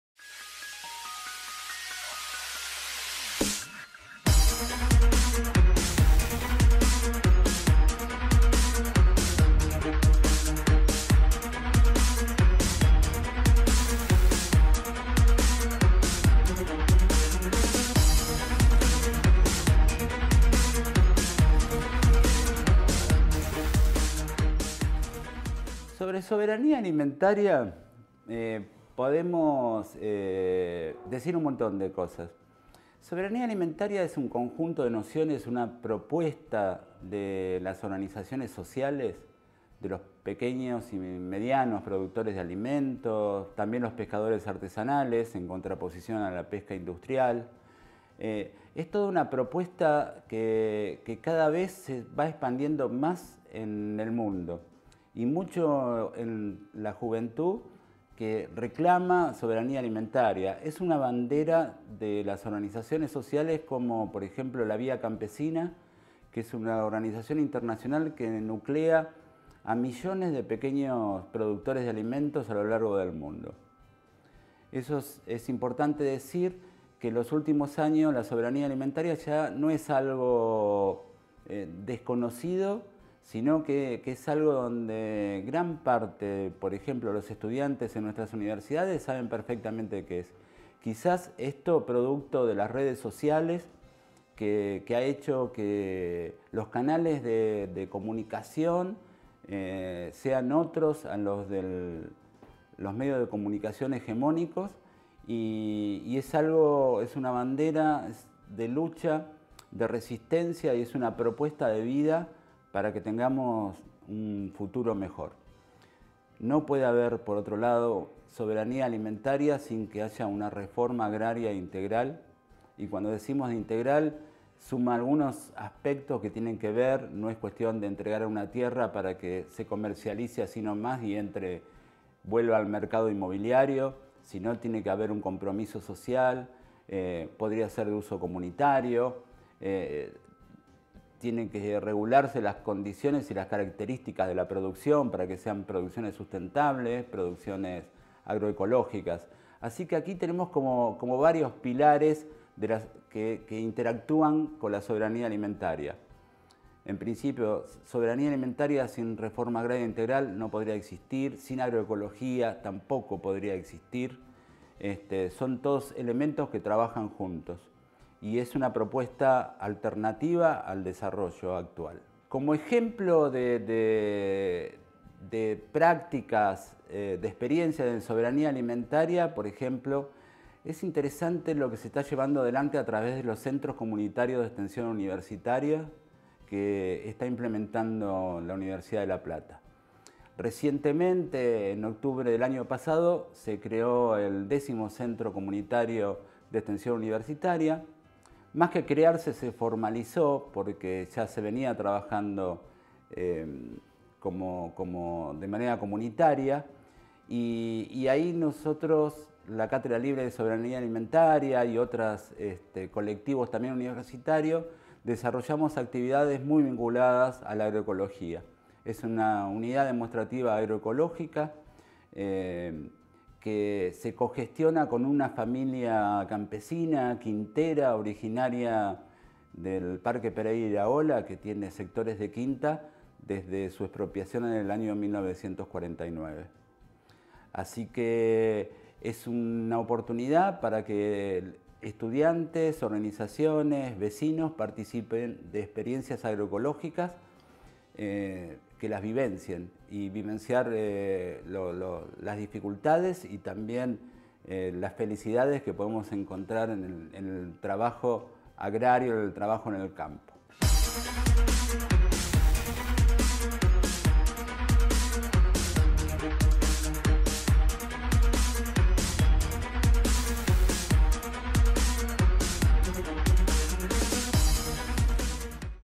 Soberanía Alimentaria: La soberanía alimentaria en los últimos años
Esta serie denominada “Herramientas para construir saberes en torno a la Soberanía Alimentaria” busca conformar un aporte a la comprensión de las cuestiones referidas al derecho humano a la soberanía alimentaria, propiciando un espacio de reflexiones críticas que permitan repensar el proyecto de desarrollo y alimentación que requerimos como sociedad, poniendo en discusión la imposición de un diseño y un modelo de agricultura pensado en la exportación y a contramano con la soberanía alimentaria de nuestro país.